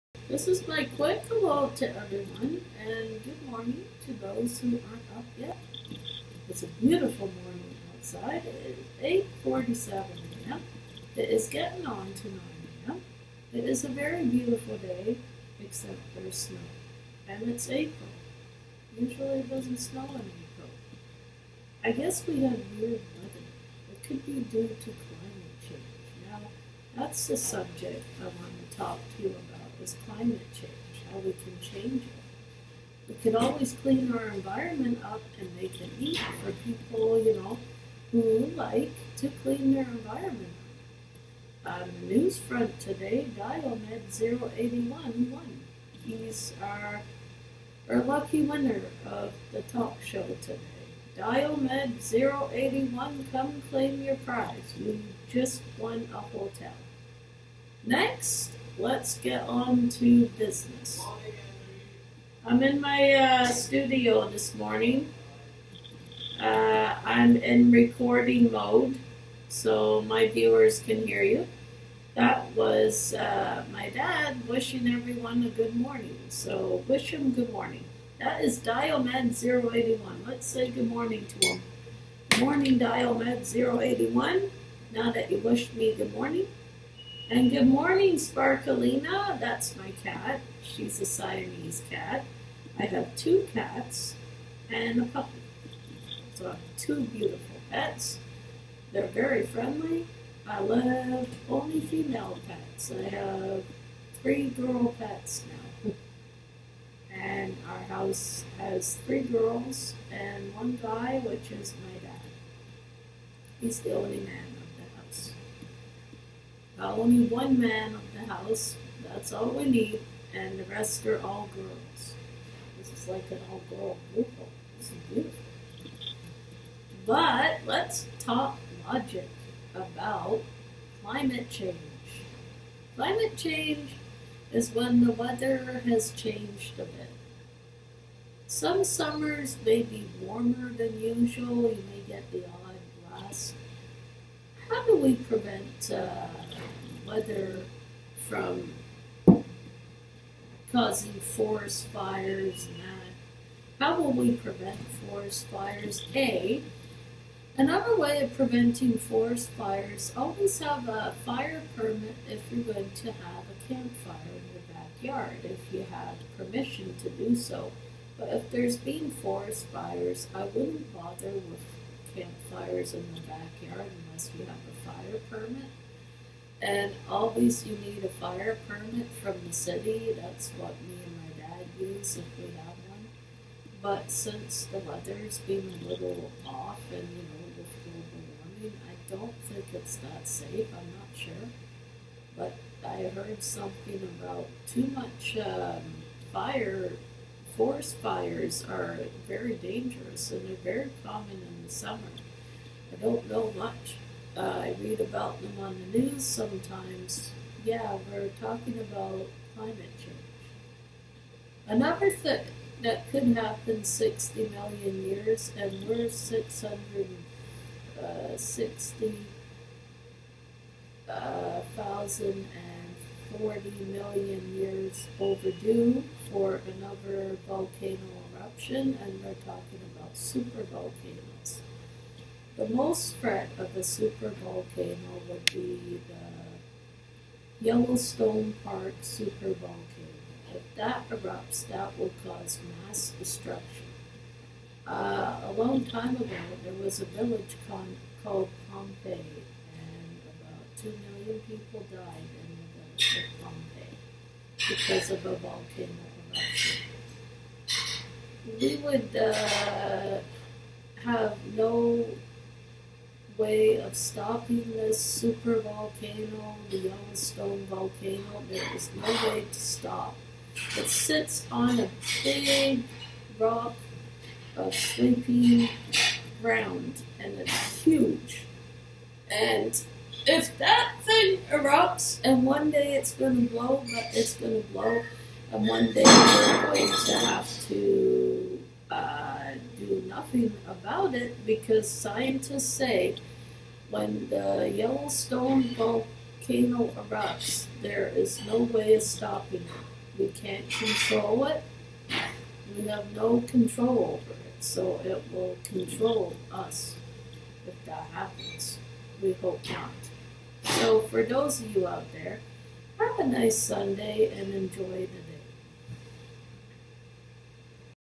enviromental talk global warming and volcanos
subject the  Yellow Stone  national Park Caldara Super Volcano